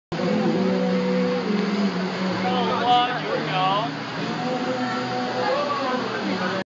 Lijiang
Lijiang old town lots of tourist very busy places.